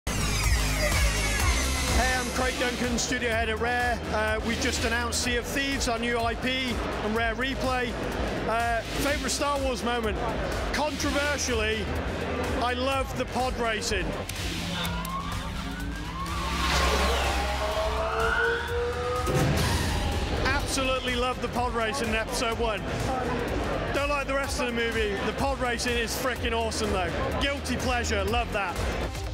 Star Wars Memories: Craig Duncan
Rare studio head Craig Duncan's favorite Star Wars Memory is from The Phantom Menace, but we'll try not to hold that against him.